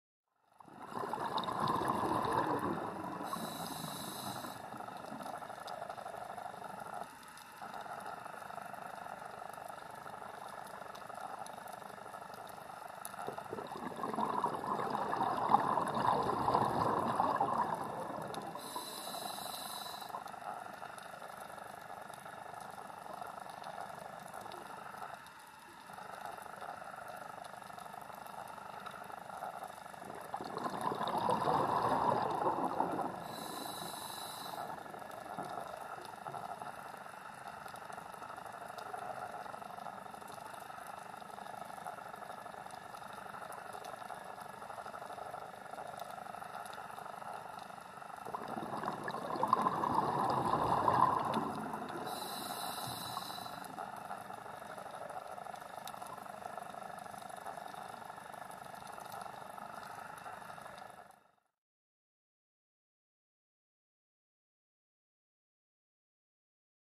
Juvenile Blue Razorfish (Xyrichtys pavo)
The juvenile of this species protects itself by adopting the colouring of a drifting piece of seaweed and mimicing the wafting movements of a leaf in the current as it swims. The species habitat is deep sand slopes such as this in Peros Banhos lagoon.